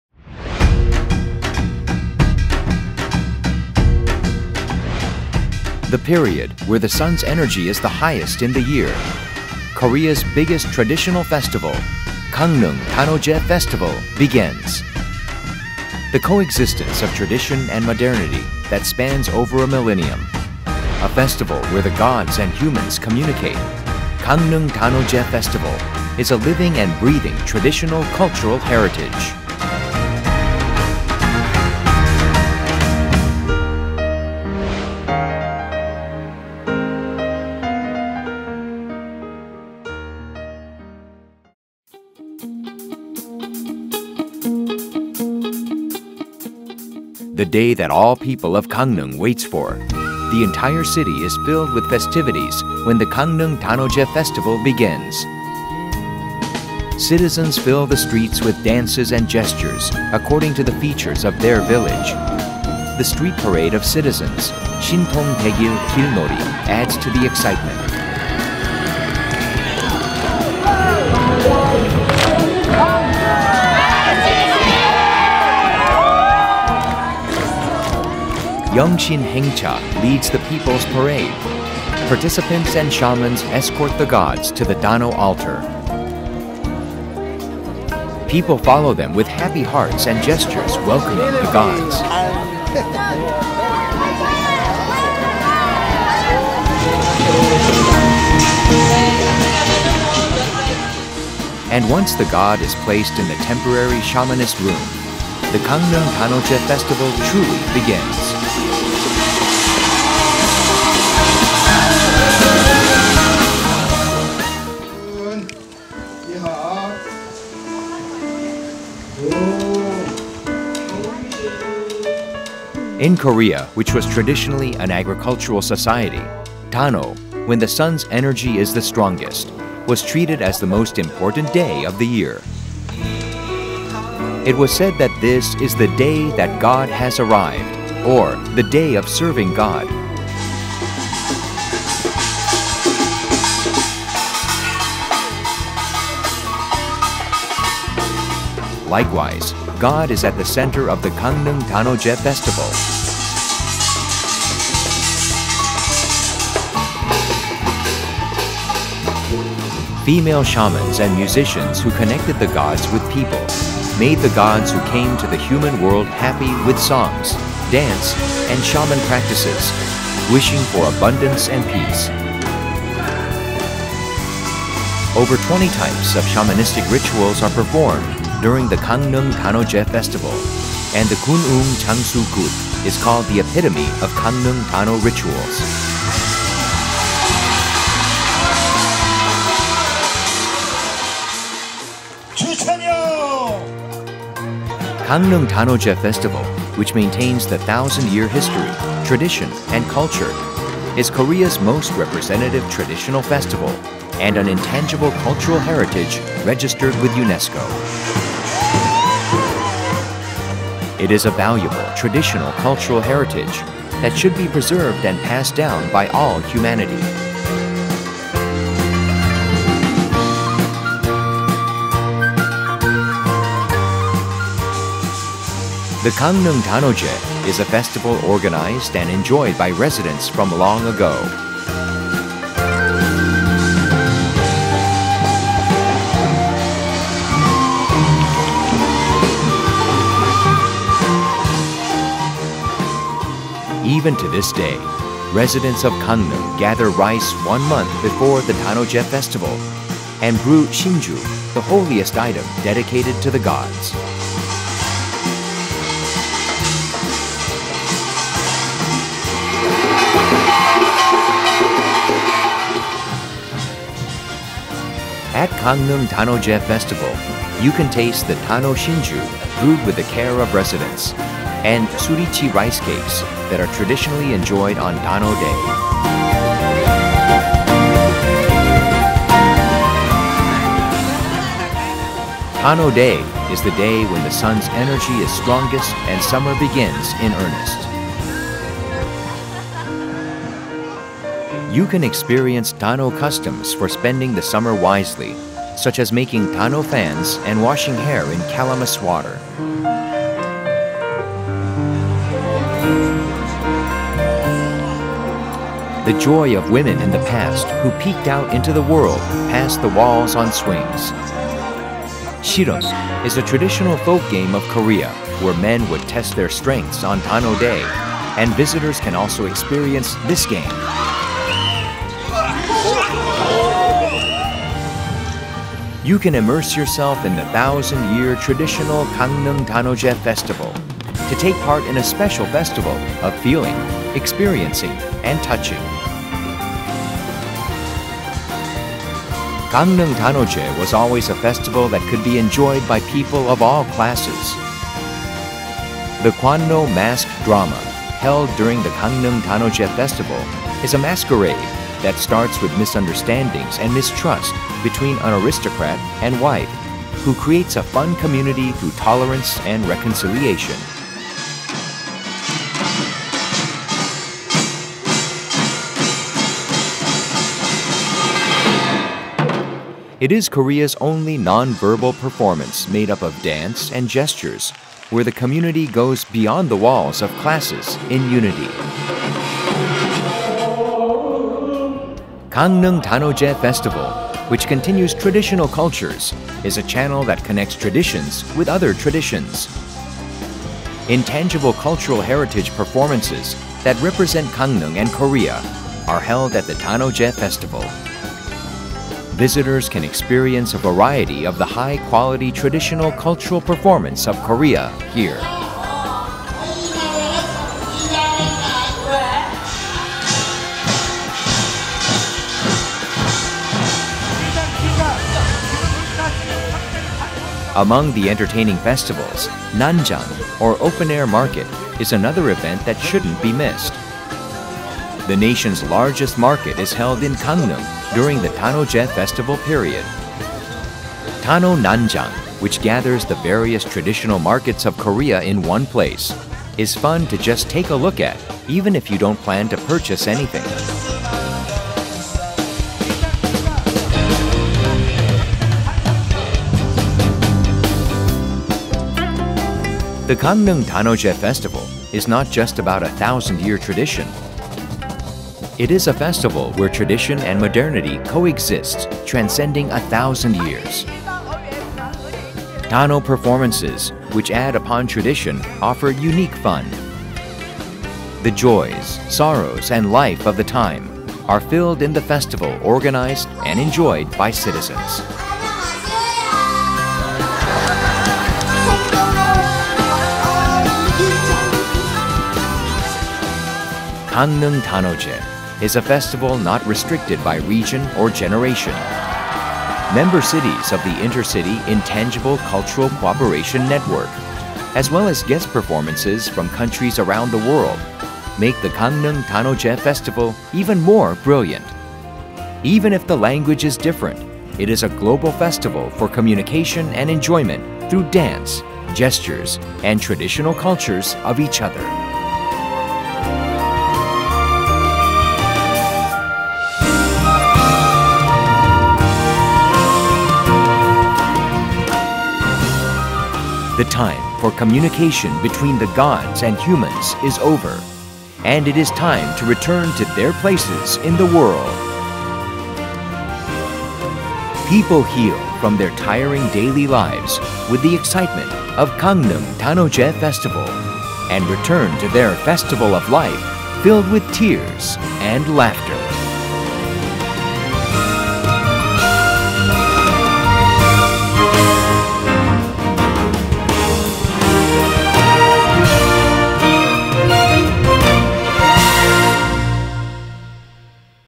Gangneung Danoje Festival PR (English)
Welcome to Gangneung Danoje!

If you visit Korea in June, don't forget to visit famous traditional festival in Gangneung.
Danoje is kind of traditional holiday, which is know for its special events.
among other province's Danoje, Gangneung Danoje is the best, UNESCO picked.

It takes only 2hours from Seoul to Gangneung.
If you have a plan for korea in June, you MUST come by Gangneung!